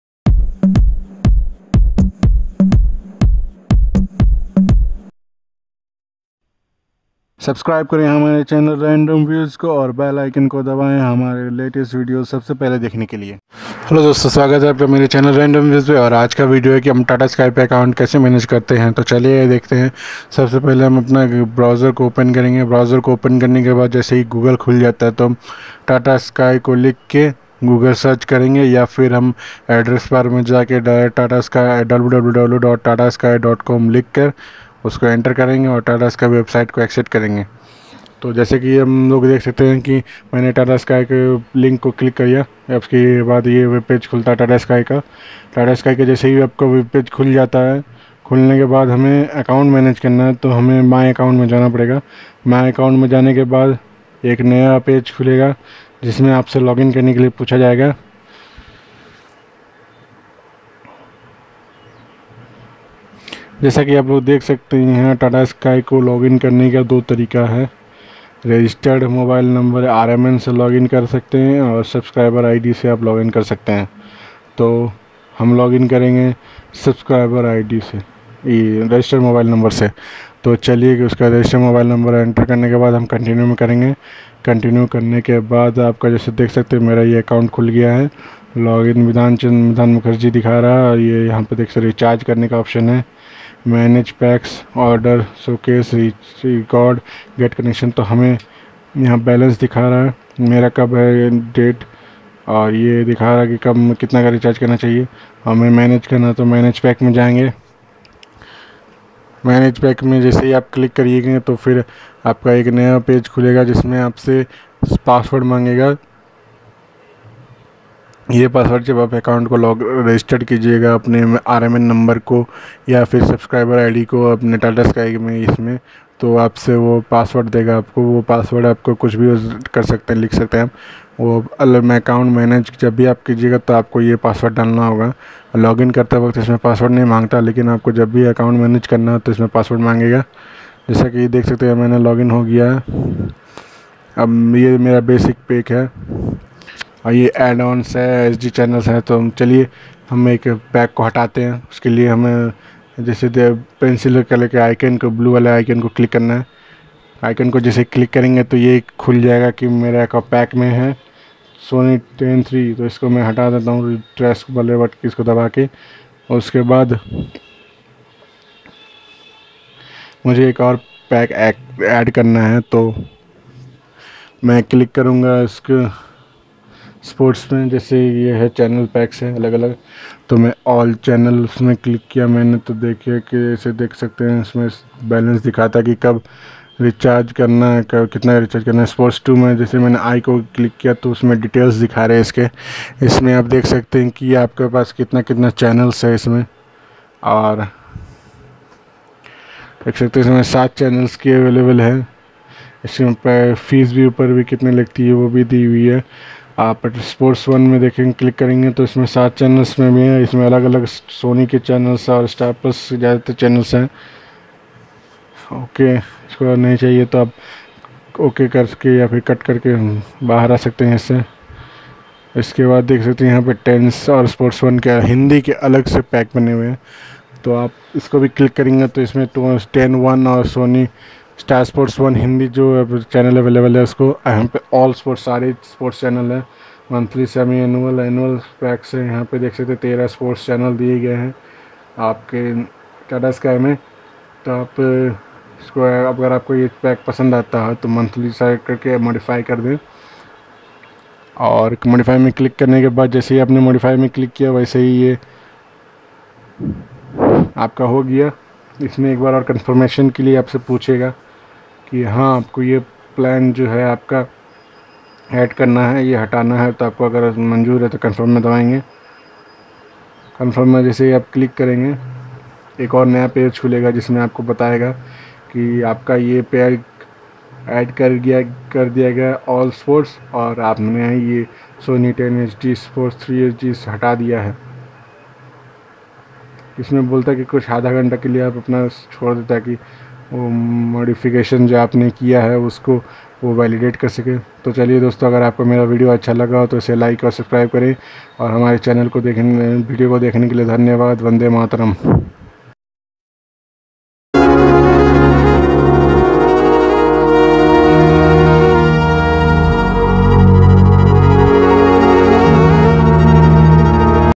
How to manage TataSky Account?
Hi Guys welcome to RandomViews. Today's video is about how to manage your TataSky account online. How to add or drop packs or channels. Keep watching for more information.